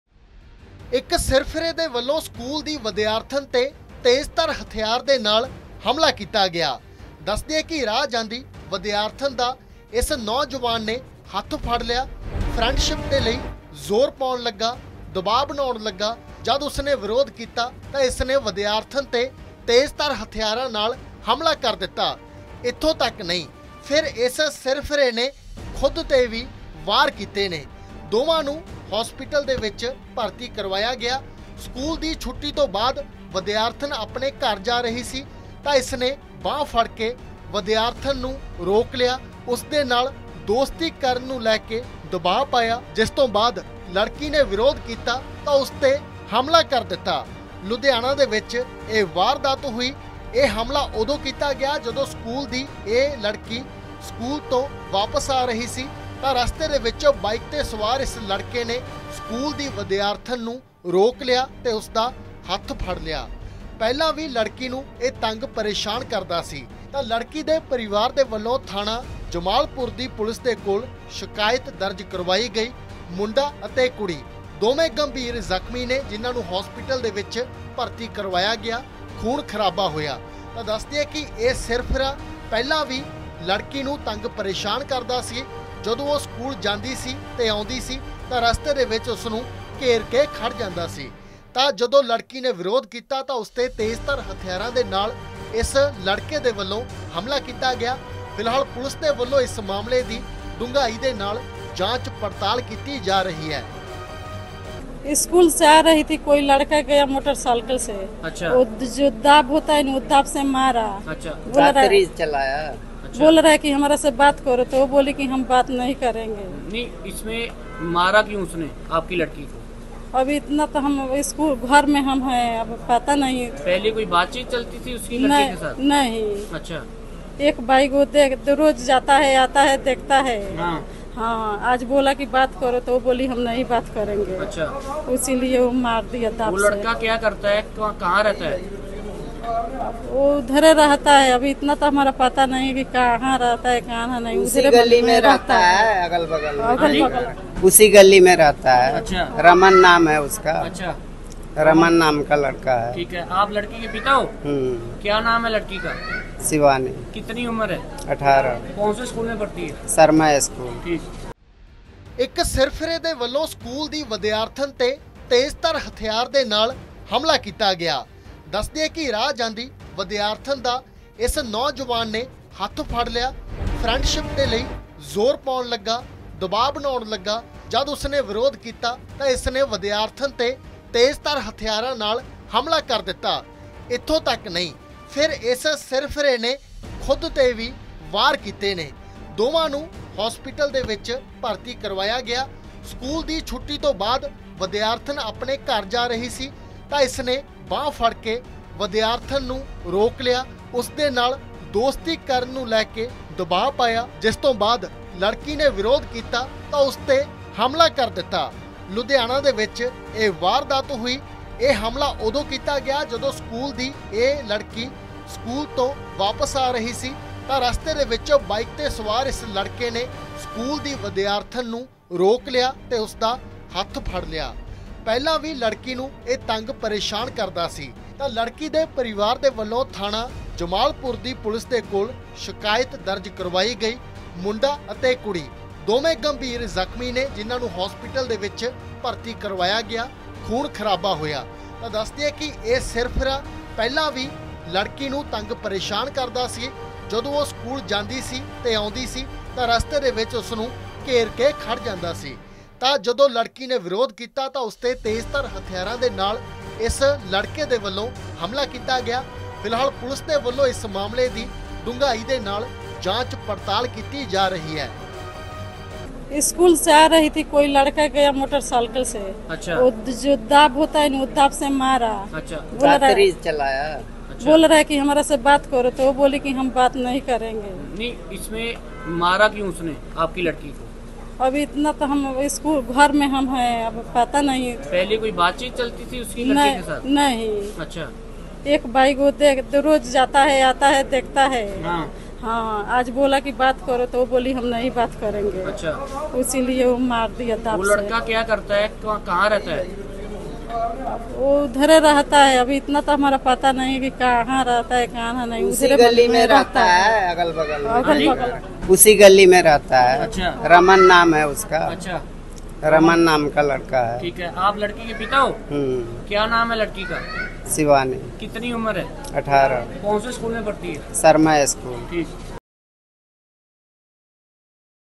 Clash In Ludhiana | ਸਕੂਲੀ ਵਿਦਿਆਰਥਣ ਨੂੰ ਰਾਹ 'ਚ ਘੇਰ ਕੇ ਨੌਜਵਾਨ ਨੇ ਕੀਤਾ ਹਮਲਾ, ਜਾਣੋ ਵਜ੍ਹਾ
ਲੁਧਿਆਣਾ ਤੋਂ ਇਕ ਮਾਮਲਾ ਸਾਹਮਣੇ ਆਇਆ ਹੈ ਜਿੱਥੇ, ਇਕ ਸਕੂਲੀ ਵਿਦਿਆਰਥਣ 'ਤੇ ਤੇਜ਼ਧਾਰ ਹਥਿਆਰਾਂ ਨਾਲ ਹਮਲਾ ਹੋਇਆ ਹੈ। ਦੱਸ ਦਈਏ ਕਿ ਦੇਰ ਸ਼ਾਮ ਲੁਧਿਆਣਾ ਦੀ ਚੀਮਾ ਕਾਲੋਨੀ 'ਚ ਰਹਿਣ ਵਾਲੀ ਇਕ ਸਕੂਲੀ ਵਿਦਿਆਰਥਣ 'ਤੇ ਬਾਈਕ ਸਵਾਰ ਨੌਜਵਾਨ ਨੇ ਹਮਲਾ ਕਰ ਦਿੱਤਾ।